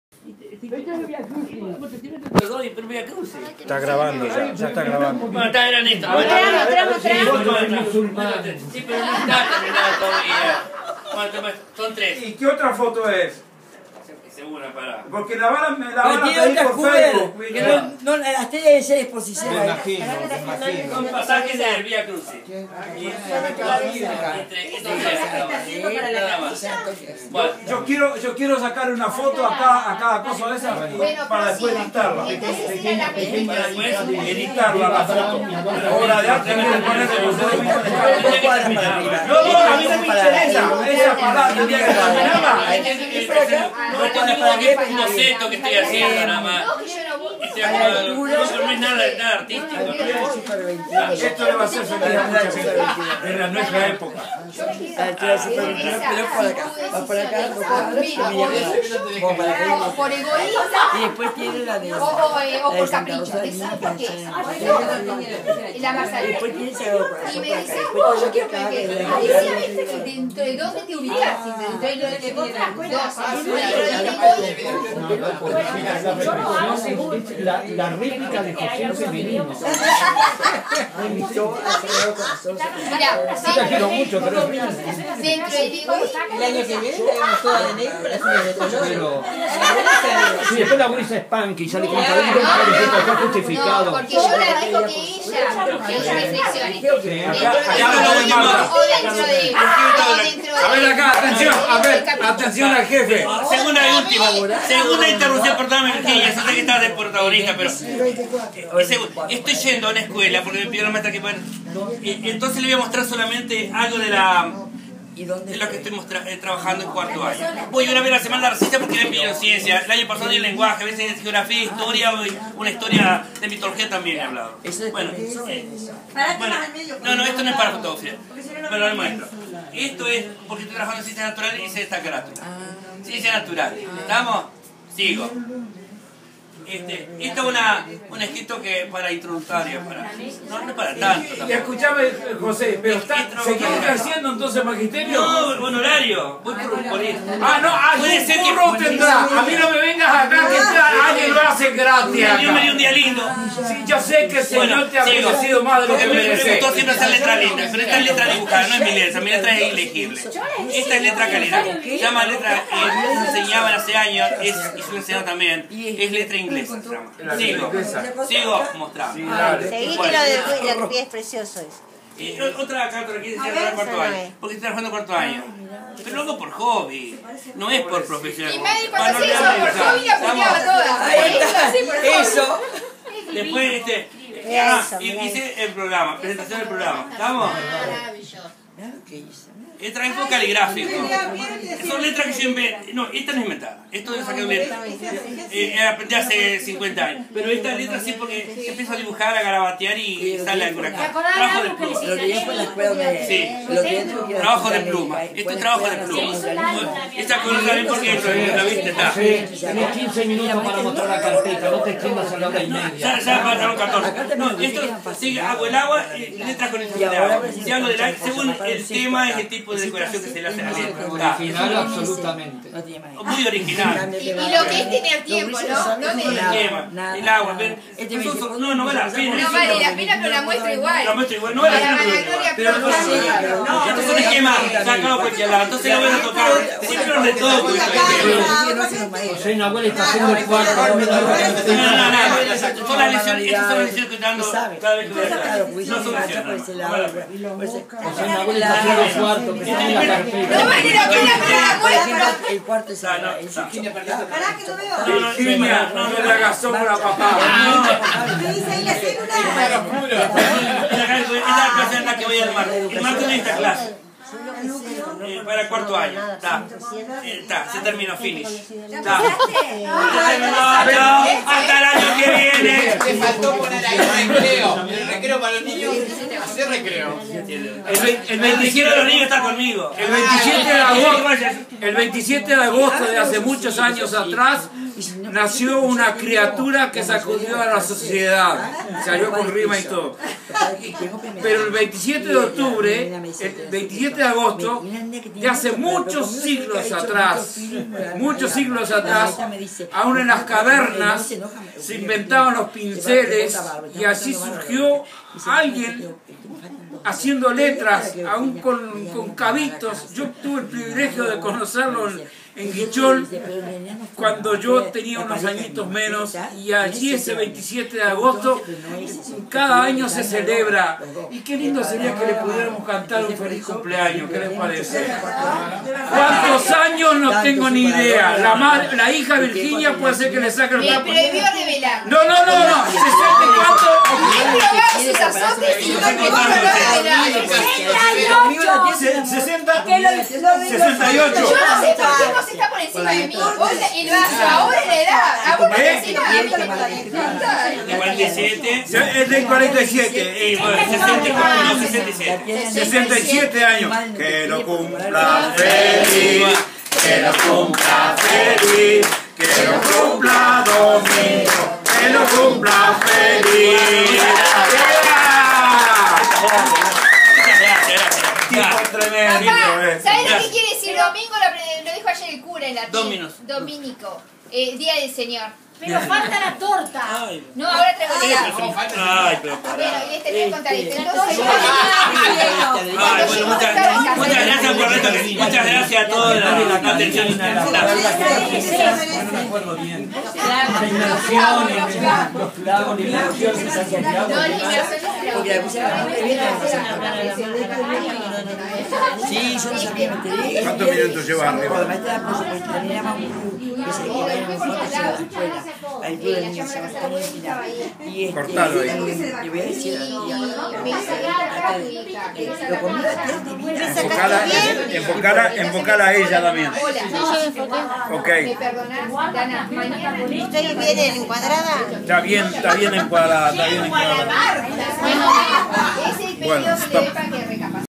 Almuerzo especial con amigos y MAESTRO LEZCANO 1
Fue un día inolvidable junto al amigo MAESTRO LEZCANO (y sus amigos) que son un regalo de Dios para mi. Grato estar felices mirando el pasado y recordando vivencias de este maestro querido por muchos en Guichon y Montevideo.